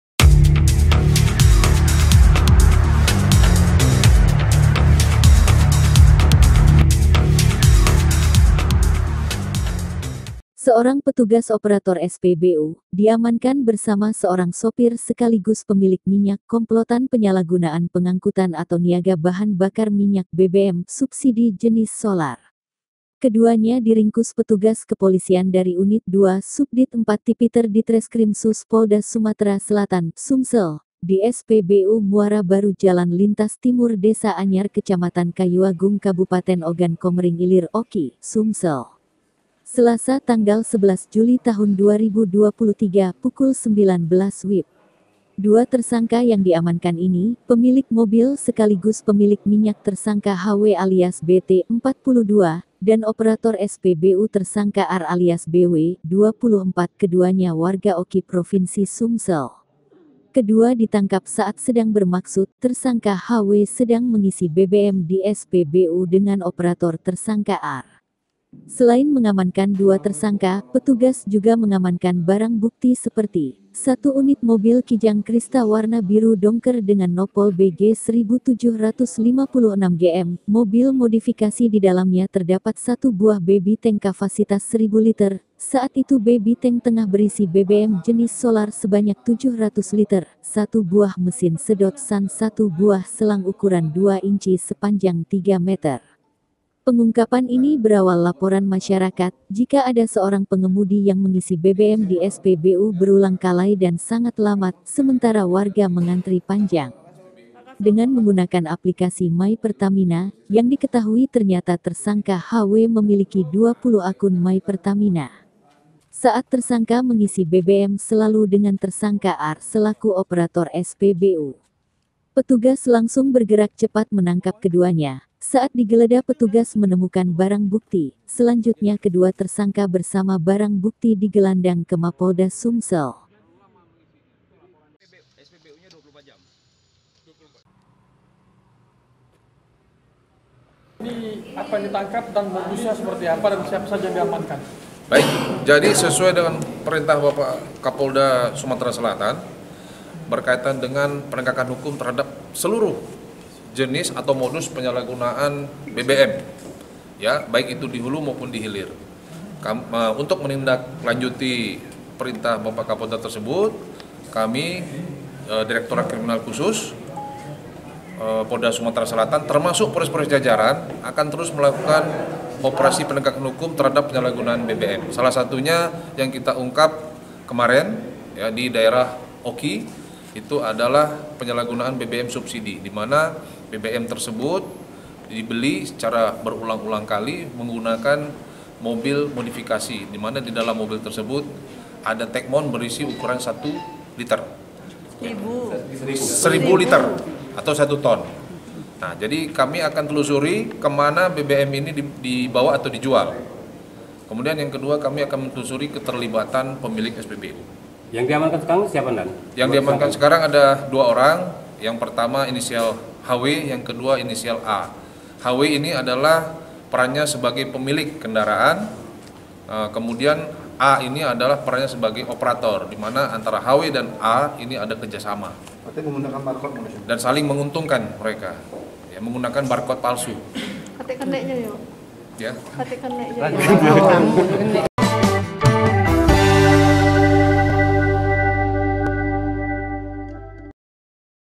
Komplotan Penyalahgunaan Pengangkutan BBM Di Ringkus Kepolisian Unit 2 Subdit IV Tipiter Polda Sumse
PALEMBANG,MEDIASRIWIJAYA – Seorang petugas Operator SPBU, diamankan bersama seorang sopir sekaligus pemilik minyak, komplotan penyalagunaan pengangkutan atau niaga Bahan Bakar Minyak (BBM) Subsidi jenis Solar.

Keduanya di ringkus petugas kepolisian dari Unit 2 Subdit IV Tipiter Ditreskrimsus Polda Sumatera Selatan (Sumsel), di SPBU Muara Baru Jalan Lintas Timur Desa Anyar Kecamatan Kayuagung Kabupaten Ogan Komering Ilir (OKI) Sumsel. Selasa (11/7) pukul 19.00 Wib.

Dua tersangka yang diamankan ini, pemilik mobil sekaligus pemilik minyak tersangka HW alias BT (42) dan operator SPBU tersangka AR alias BW (24), keduanya warga OKI Provinsi Sumsel. Kedua di tangkap saat sedang bermaksud, tersangka HW sedang mengisi BBM di SPBU dengan operator tersangka AR.